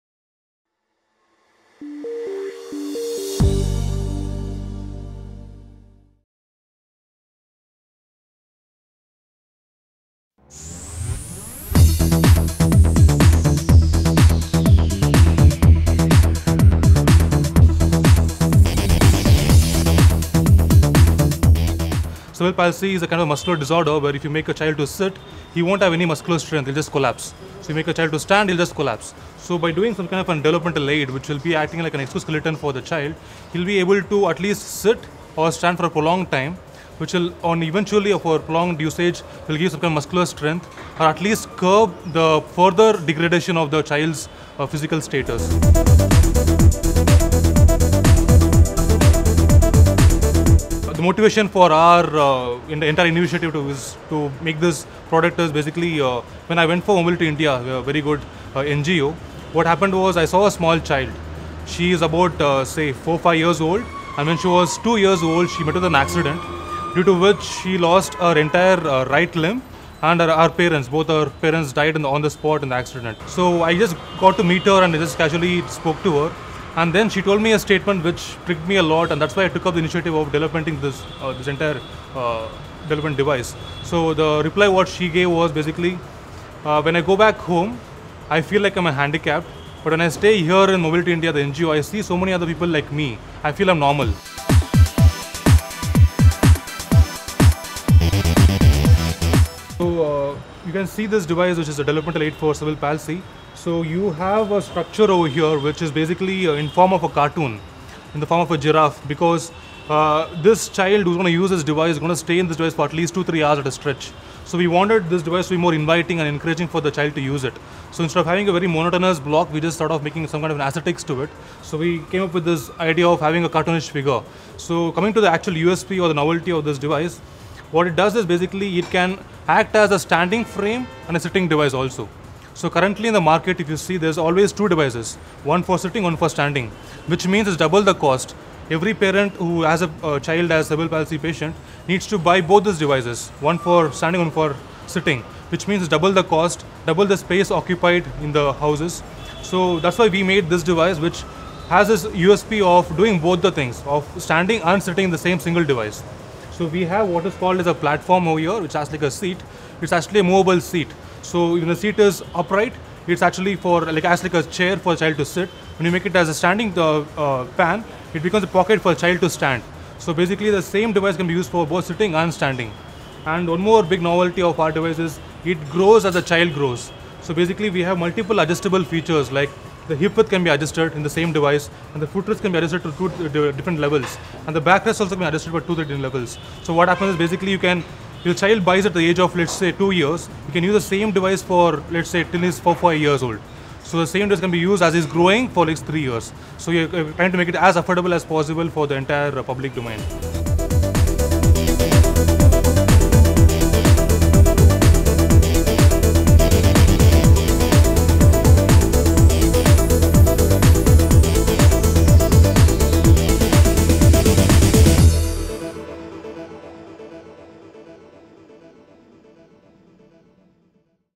Same Device Helps Cerebral Palsy Children Sit as well as Stand
Riyaz H, Trivikram, Indra Sena Reddy, Shiva Kumar, and Praveen have come up with a low cost, pre­fabricated device for children with cerebral palsy which can be used by them to sit as well as stand. With an easily-adjustable frame, this device will be useful for children between 2-4 years old.